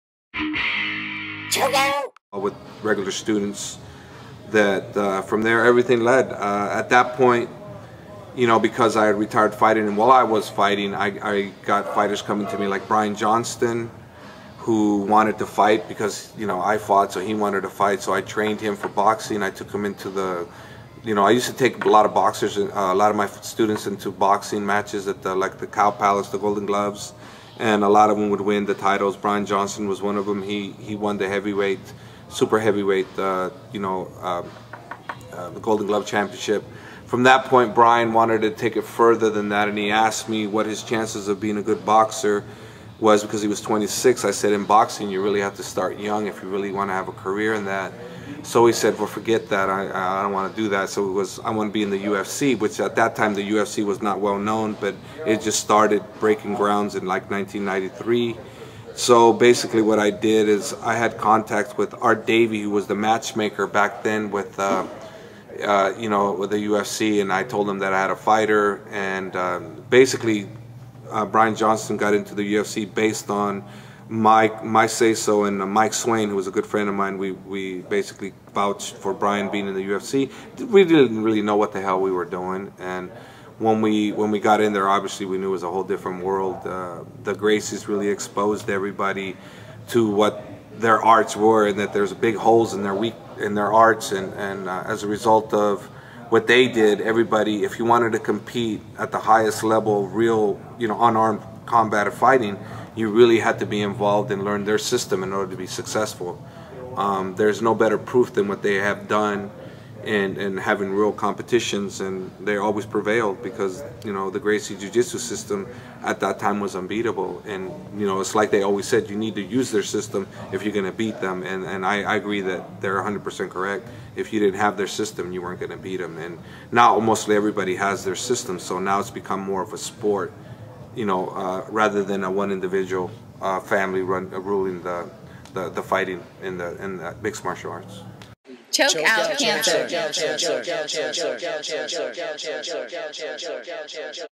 History of AKA into MMA and UFC
ChokeOuT Cancer got a history lesson from AKA owner Javier Mendez.  How AKA got started, got into MMA and the UFC.  AKA trains UFC champs like Cain Velasquez, BJ Penn and Frank Shamrock.  Strikeforce champs like Daniel Cormier and Luke Rockhold  are also AKA team-mates.  This is not your ordinary gym.